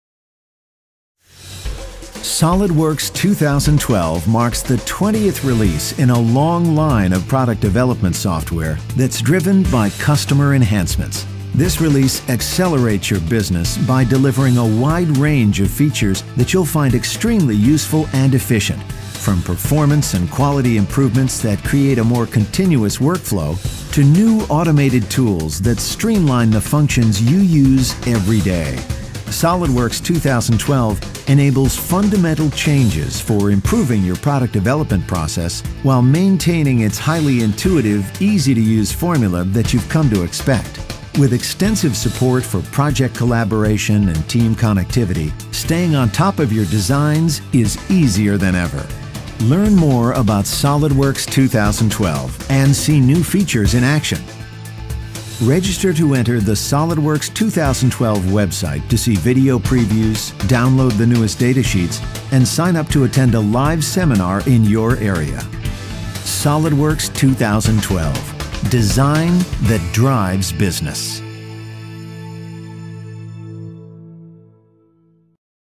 SolidWorks 2012 - Design that drives your business
The benefit is simple—SolidWorks® 2012 will help you design faster and do your job better.

With SolidWorks 2012, you can work more efficiently and have the data you need to make better design decisions throughout your product development process.